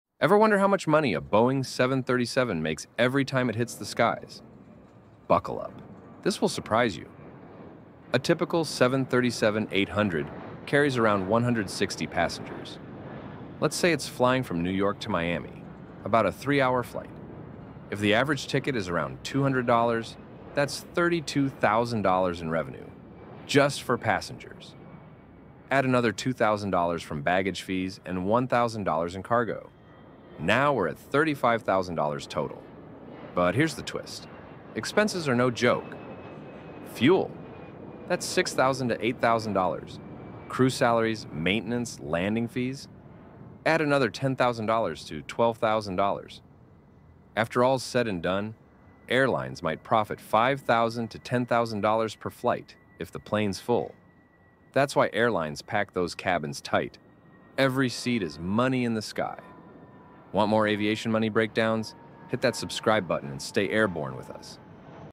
How Much Does a Boeing 737 Make Per Flight? (The Surprising Truth 💸✈️)
Every time a Boeing 737 takes off, it could be earning tens of thousands of dollars — or losing money fast. 💰✈️ In this Short, we break down exactly how much airlines make per flight, from ticket sales to fuel costs, crew salaries, and more. The numbers will shock you. 😲

🚀 Ever flown on a 737? You might never look at it the same again.